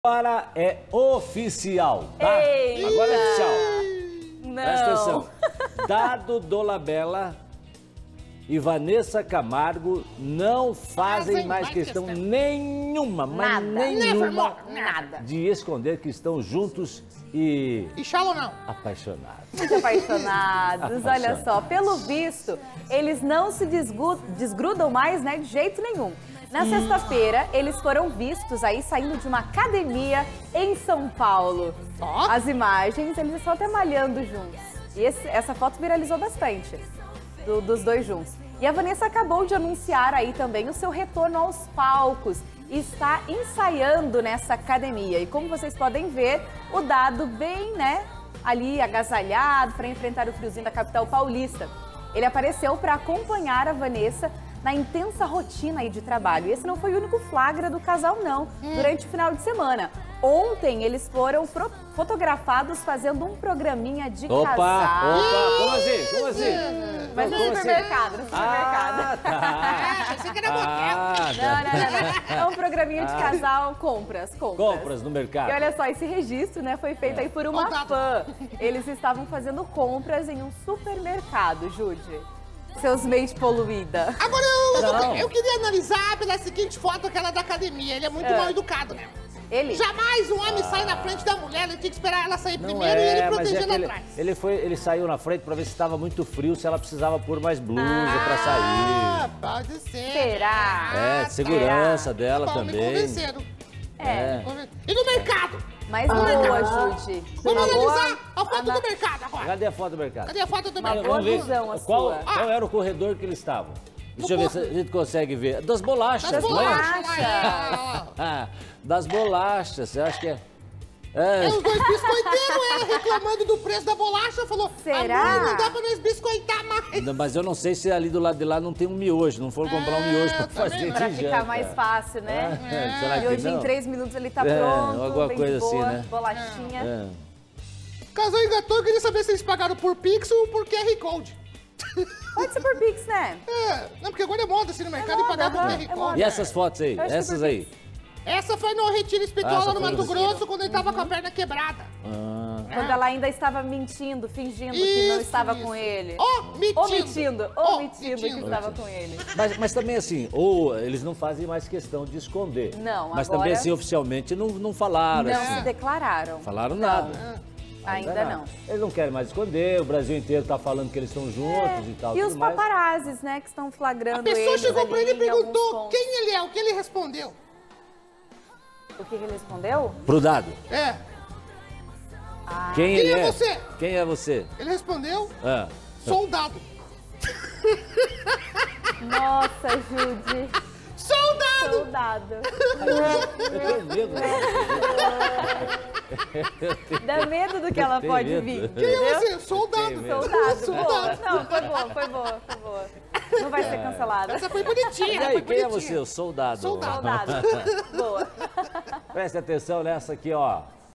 Dado Dolabella e Wanessa Camargo não fazem mais questão de esconder a relação
Dado Dolabella e Wanessa Camargo não fazem mais questão nenhuma de esconder que estão juntos e apaixonados!